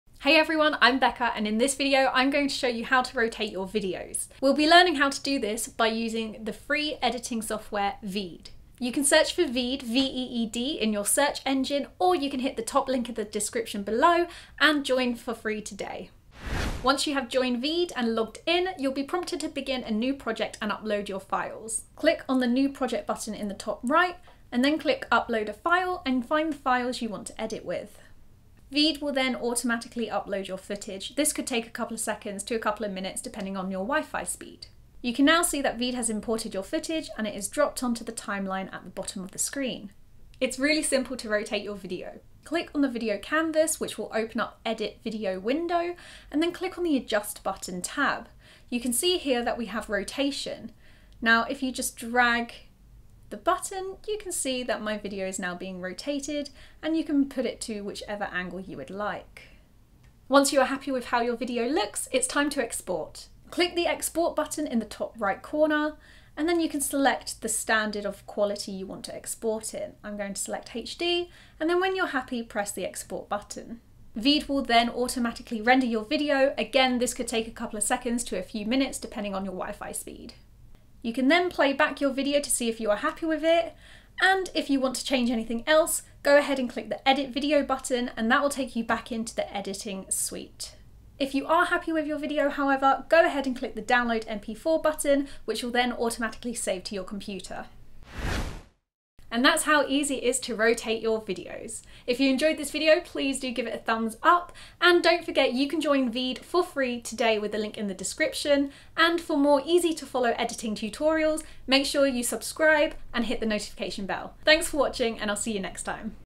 How to Rotate Videos
Learn how to quickly rotate your videos!

In this short video, we will cover how to rotate a video, using Veed, the online editing software.

Join/login to Veed and begin a new project. Upload the audio file you wish to use.

Click on the video canvas which will open up the Edit Video feature. Then click Adjust.

Under the Rotation option, click and drag the button to whichever degree you wish to rotate your video too.

When you are finished rotating your video, click the ‘export’ button in the top right corner, and select which resolution you want to export in. Then hit export.

Veed will then render your video, this may take a few minutes depending on your wifi and video length.

When it has exported you can hit the Download button and the file will be saved straight to your computer.

How to Rotate Videos Step by Step:

00:00 Intro
00:08 Join Veed
00:40 How to Rotate a Video
01:00 How to Export a Video
01:39 Outro

Good luck!

- 4k video quality
- Longer videos
- Custom fonts

Becca & VEED.IO